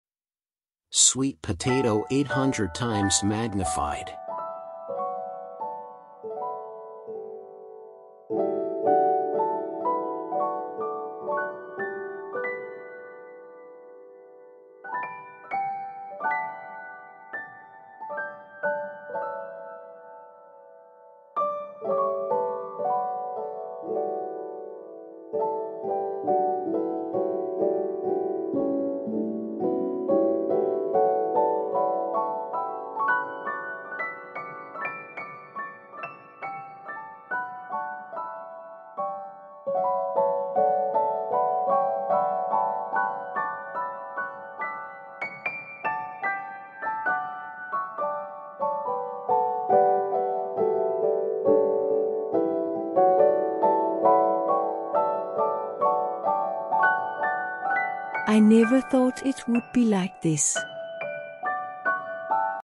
Sweet Potato Zoomed 800 times Magnified and see by Microscope
The Amazing Microscope World